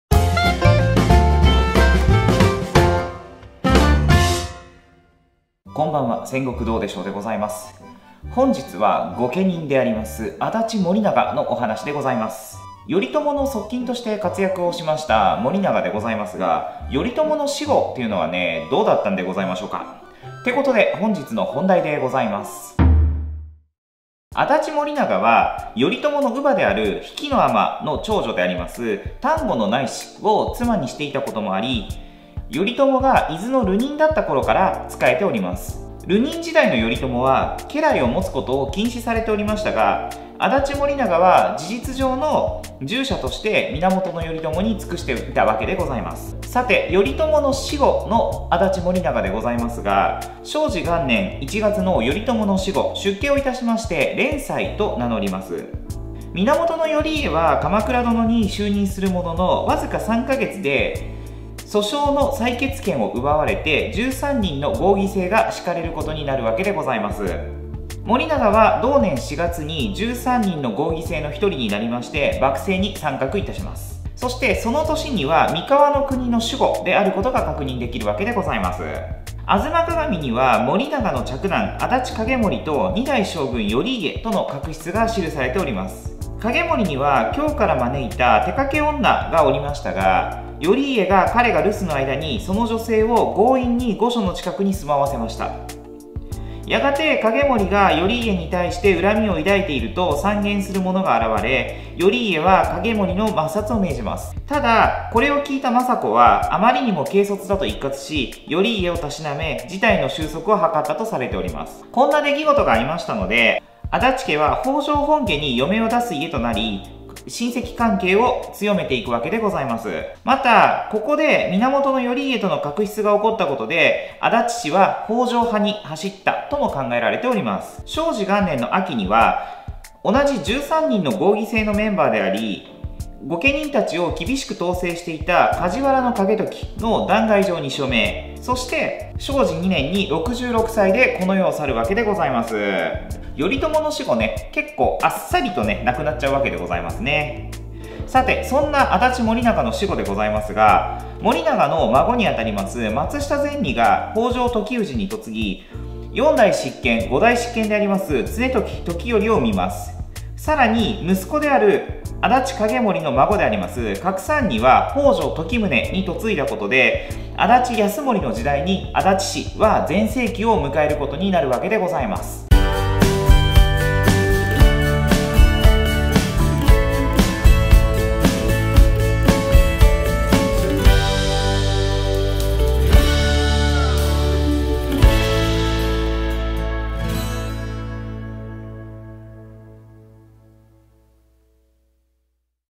【鎌倉殿の13人】頼朝死後の盛長と安達氏は！？
頼朝の側近として活躍した安達盛長。
頼朝の死後はどうだったのでしょうか！？